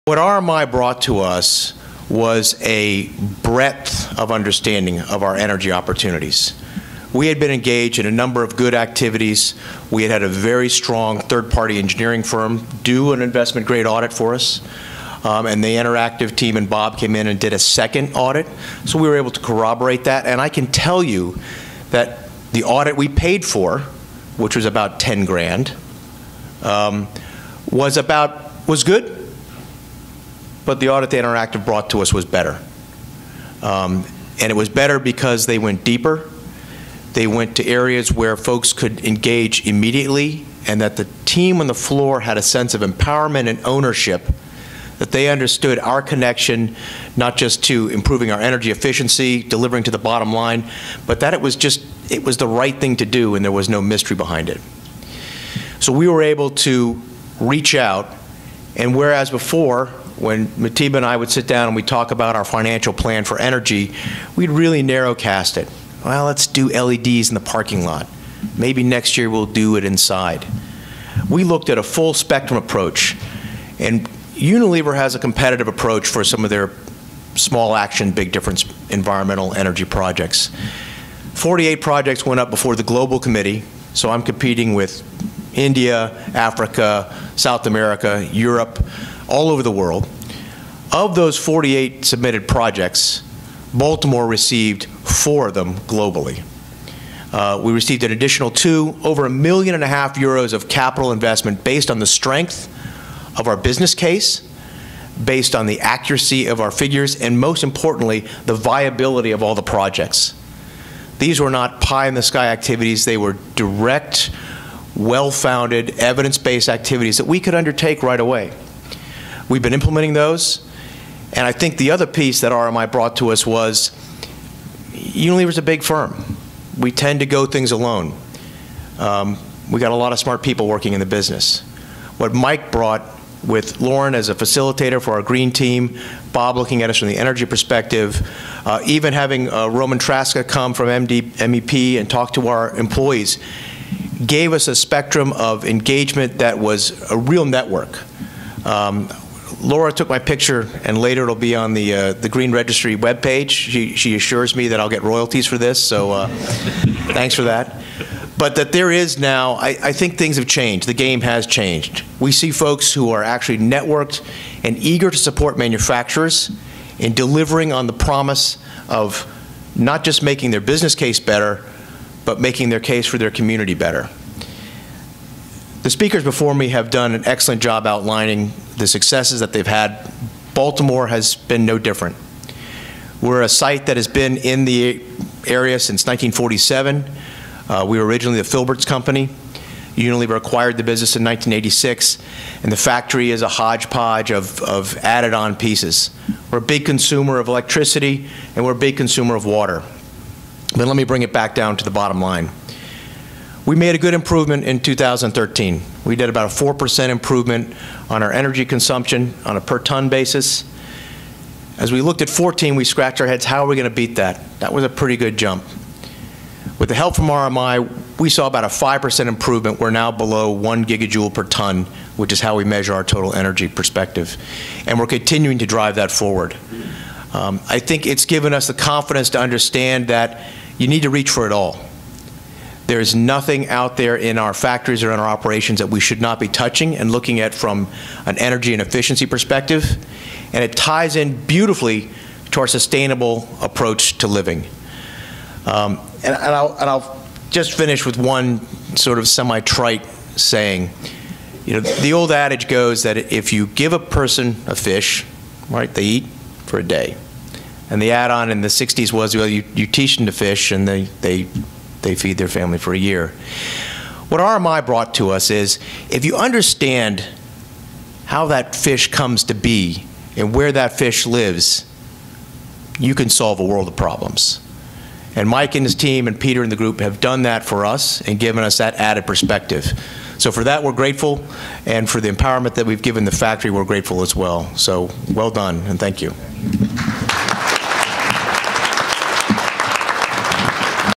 Jonathan Jett-Parmer, Factory Director, Unilever Baltimore at RMI Feb 10, 2015 Energy Forum
Unilever Baltimore is a recent participant in RMI's Energy Efficiency Program which is geared towards waste reduction and helping to create new revenue streams for Maryland manufacturers.